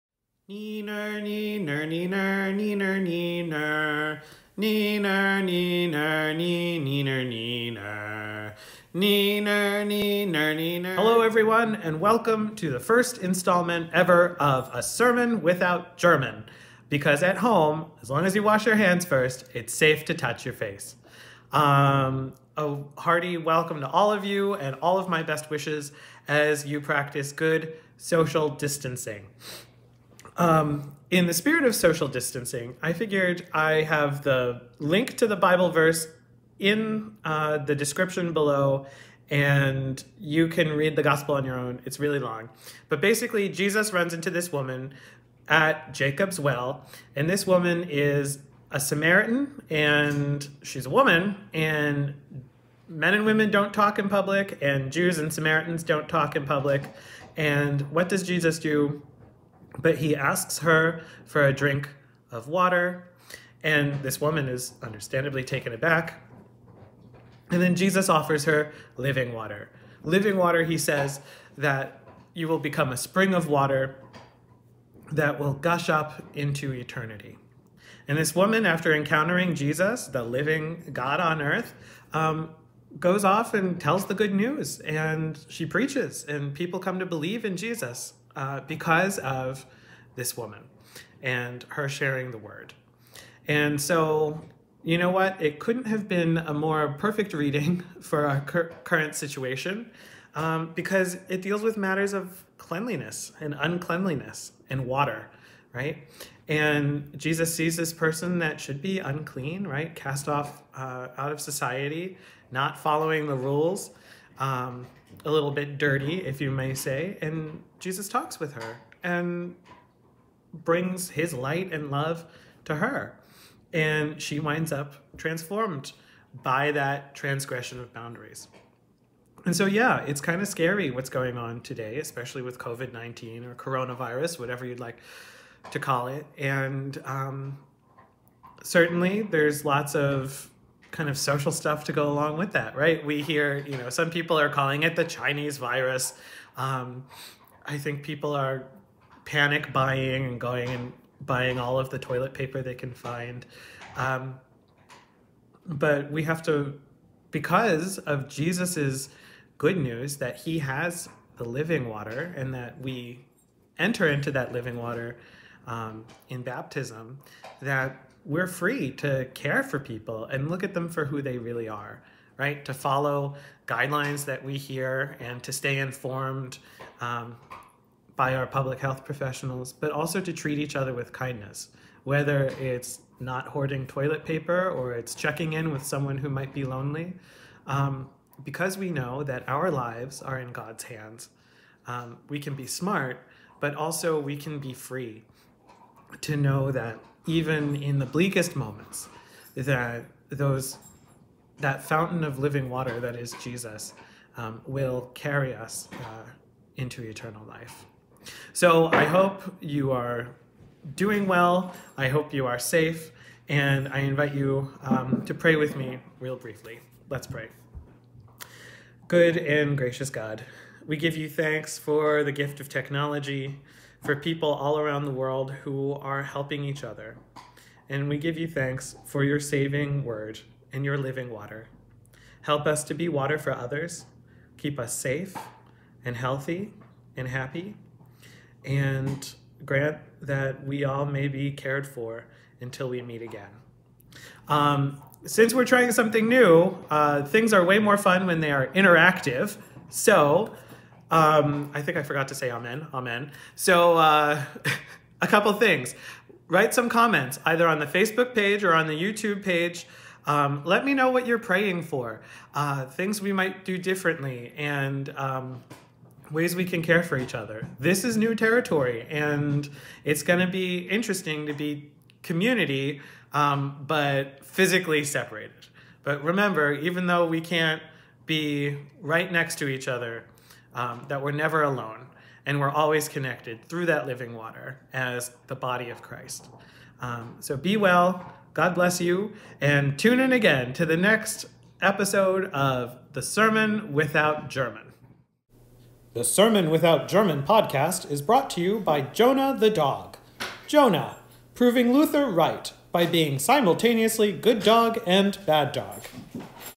Sermon without Germin' #1: Well, Hello Jesus!
Pastor Joseph Graumann shares a few thoughts about Jesus and the Samaritan Woman at the Well!
Any prayer concerns? Leave them in the comments section below!